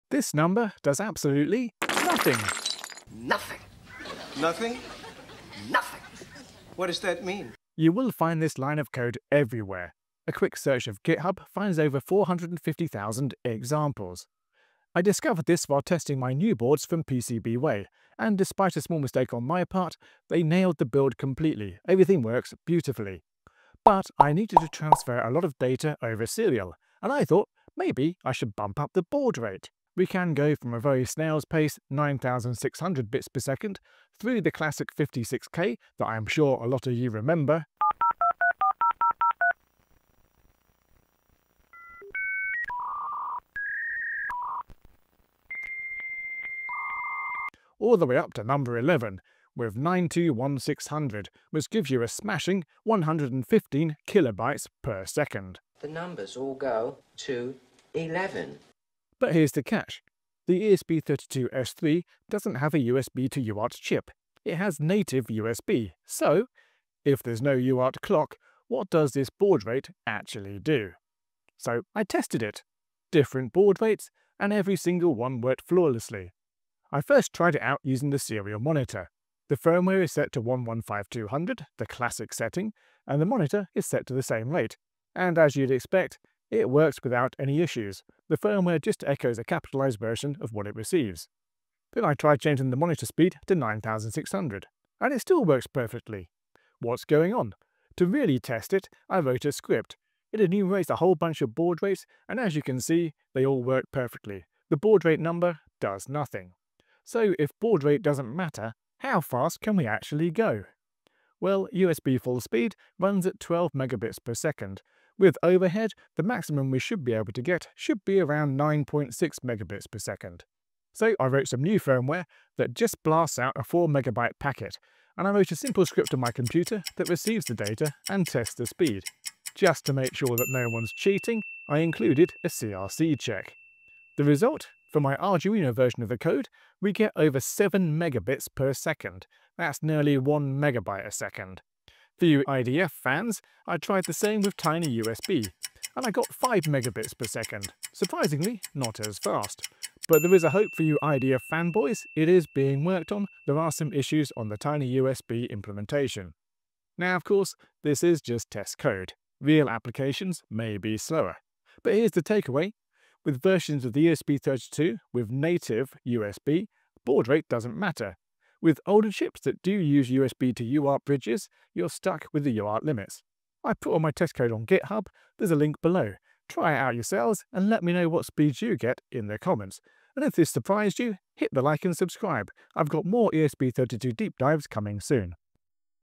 This Number Does Nothing… when you have native USB
for the fantastic PCBs and SMT assembly.

I was testing out my new boards and sending a lot of data over serial - and I started thinking - hang on a minute - this amount of data is much higher than the serial baud rate I set...